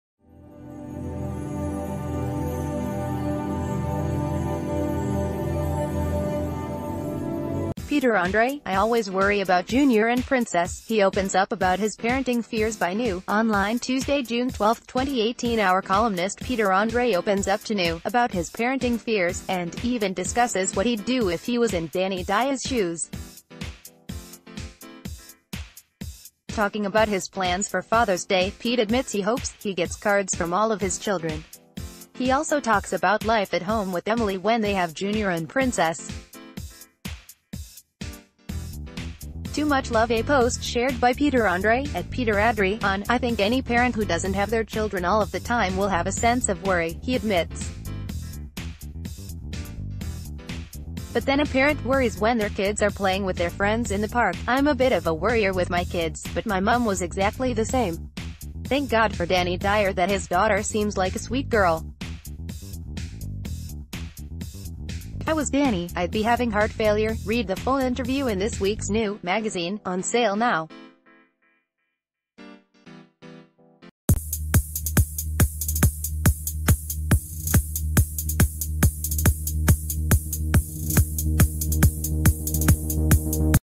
Breaking News | Pete: "I always worry about Junior and Princess!"
Peter Andre: "I always worry about Junior and Princess!" He opens up about his parenting fears BY new! online Tuesday, June 12, 2018 Our columnist Peter Andre opens up to new! about his parenting fears - and even discusses what he'd do if he was in Danny Dyer's shoes... Talking about his plans for Father's Day, Pete admits he hopes he gets cards from all of his children! He also talks about life at home with Emily when they have Junior and Princess. Too much love A post shared by Peter Andre (@p...